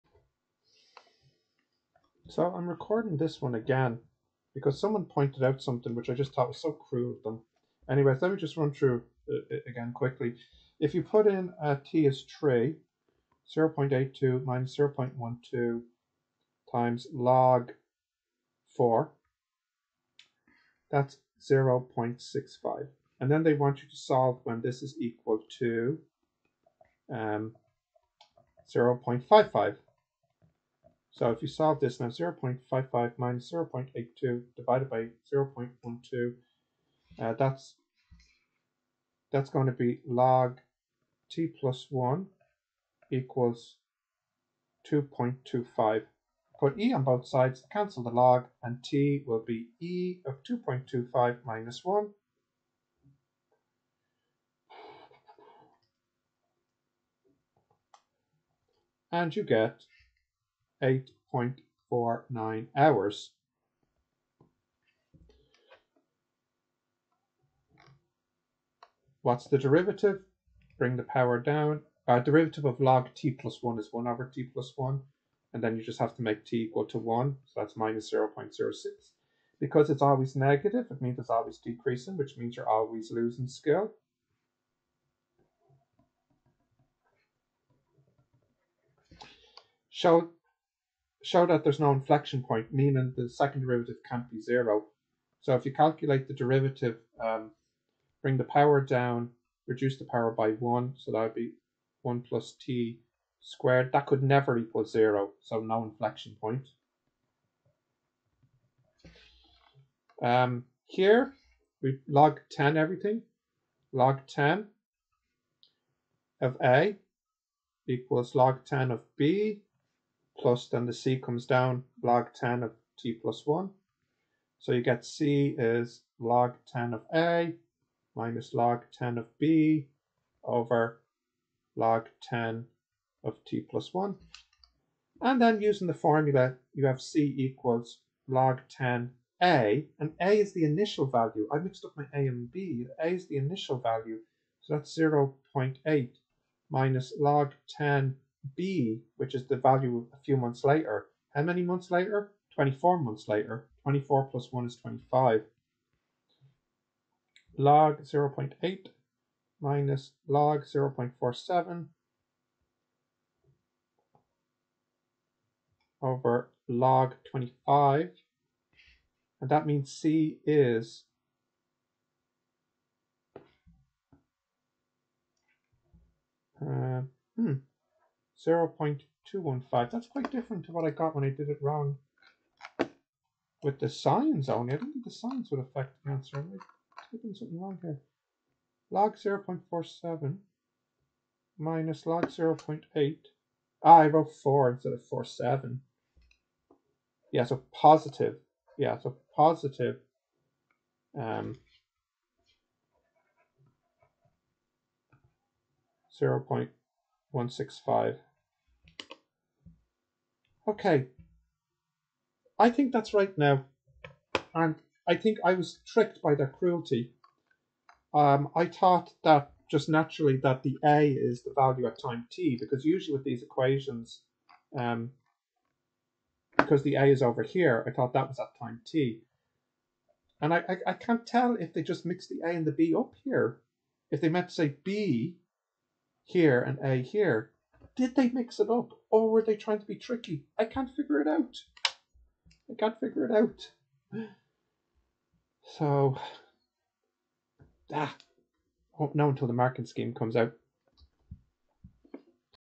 2022 Leaving Cert Higher Level Maths Paper 1 Question 10 [LIVE-ish]
This is my SPEEDRUN attempt of getting through ALL of today’s Maths Exams (JC and LC at O and H). Is it perfect? No. Might there be mistakes? Yes. But at least there are some answers out there for student to look at and talk about.